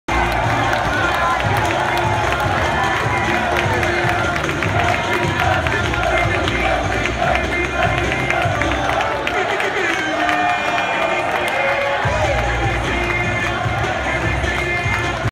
Furman's 6th man
Furman basketball team upsets Tennessee Chattanooga, then coaching staff and team swarms and celebrates with student section.
Played before students returned from winter break, men's lacrosse team did an outstanding job in filling student section and creating energy-filled auditorium.